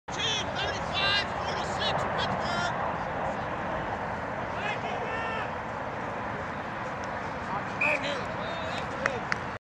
Scioto vs Pittsburgh 10-Oct-2009 Second Half IX.MOD
Situation:  50th min, Scioto (Black) 15, Pittsburgh 19
(Result:  Scioto Valley 15, Pittsburgh 19)